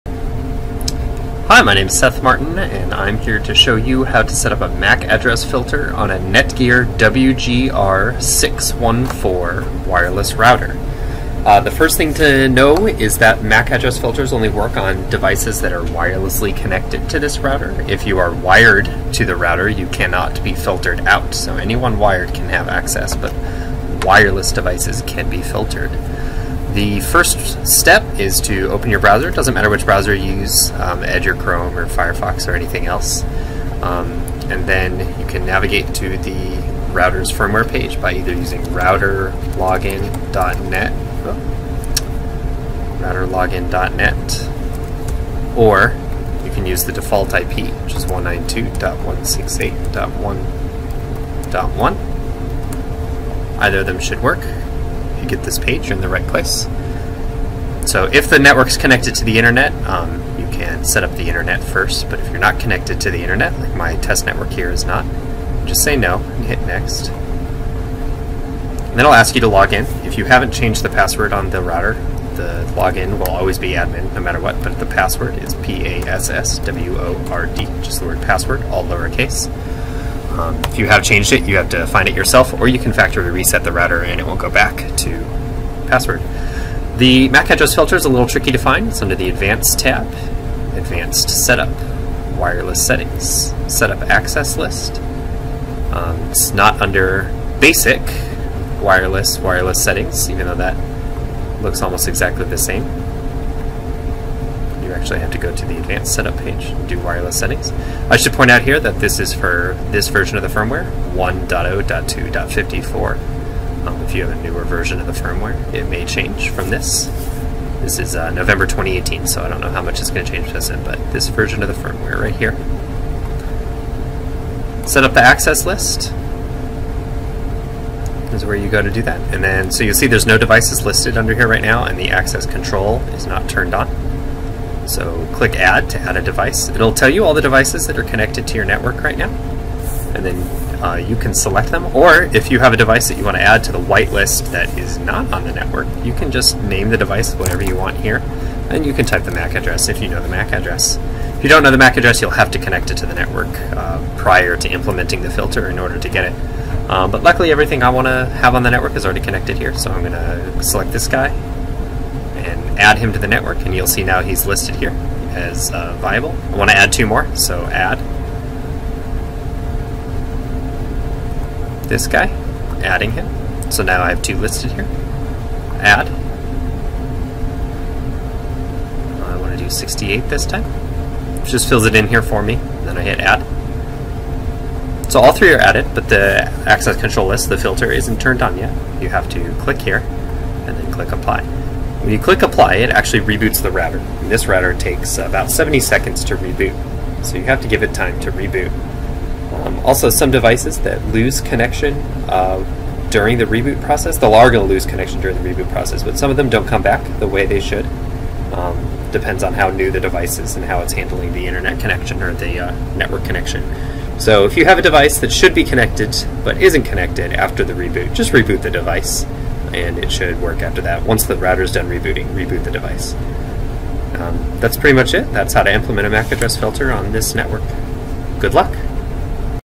How to implement a MAC address filter on a Netgear router current December 2018
Netgear's instructions for implementing the MAC filter on their Genie firmware for their routers is outdated, and doesn't show the exact right steps. This video shows you how to log in, find the access list, select the devices that should have access, and trigger these settings on your Netgear router running Genie version 1.0.2.54.